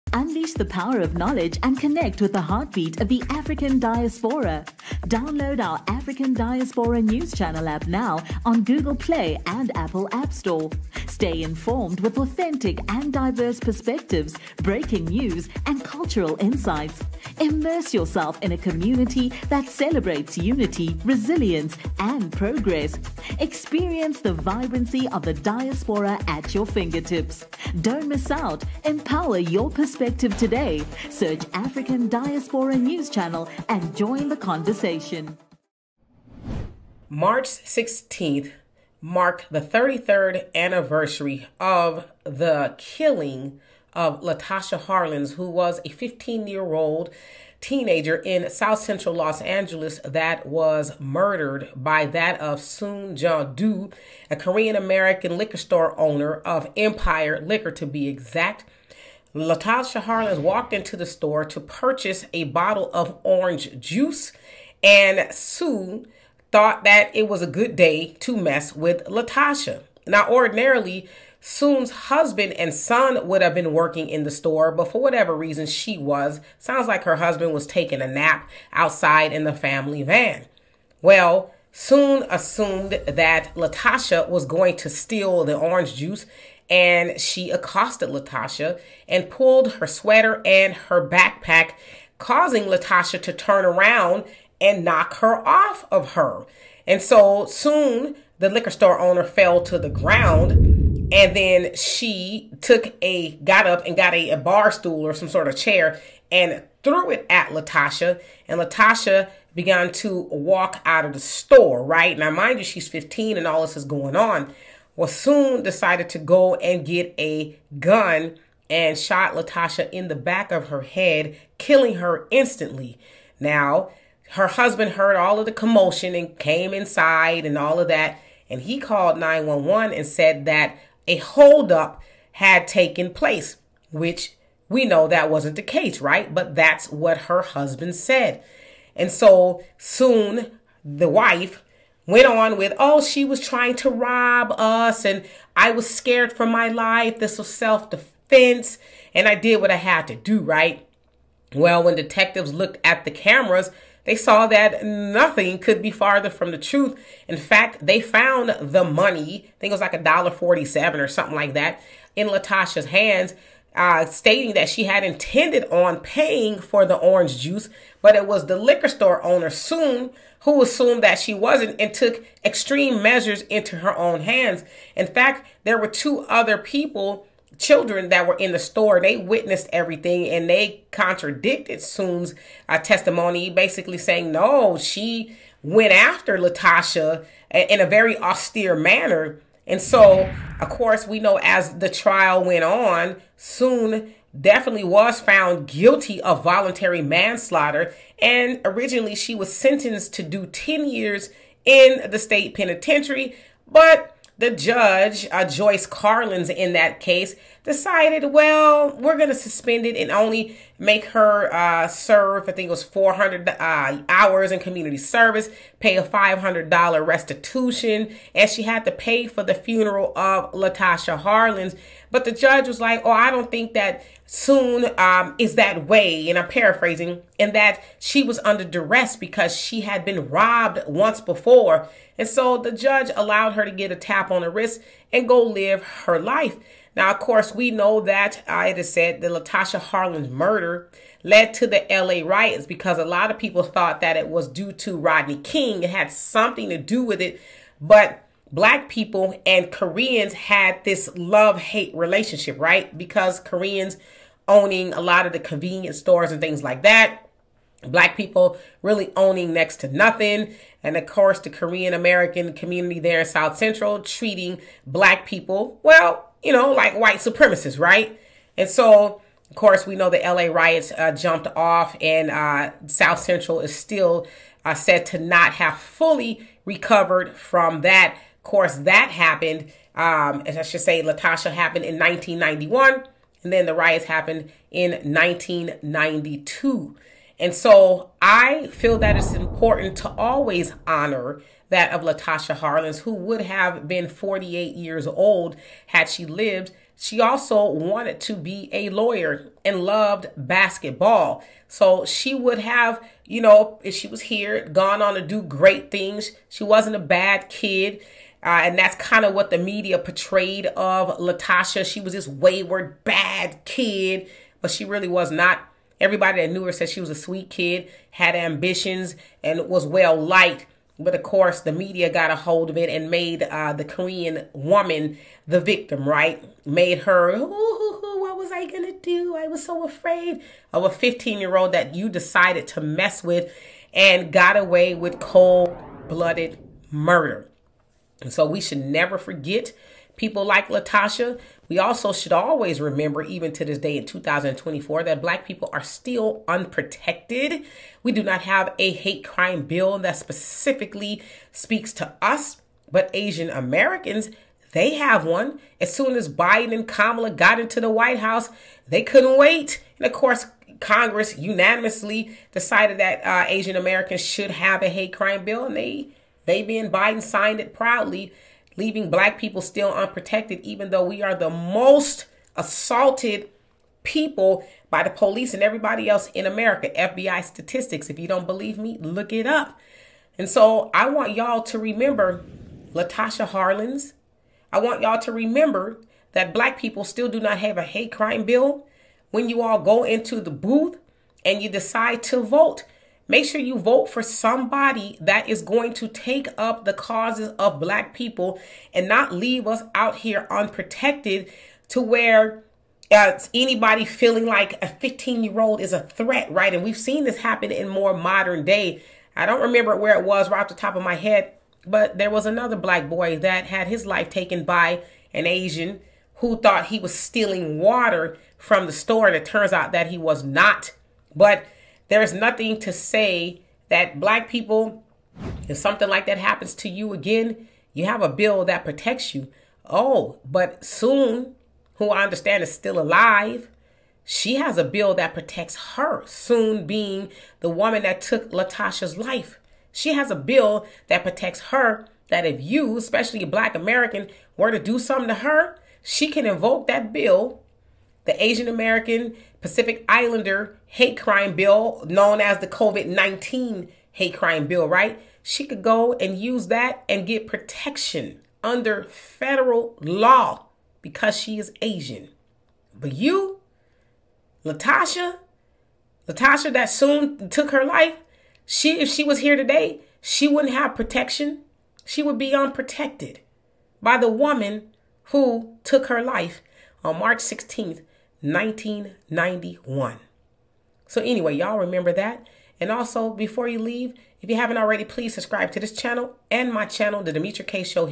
Remembering Latasha Harlins 33 Years After Her Life Was Cut Short By Soon Ja Du In South Central LA
Demetra Kaye reports on remembering Latasha Harlins 33 years after her life was cut short by Soon Ja Du in South Central Los Angeles.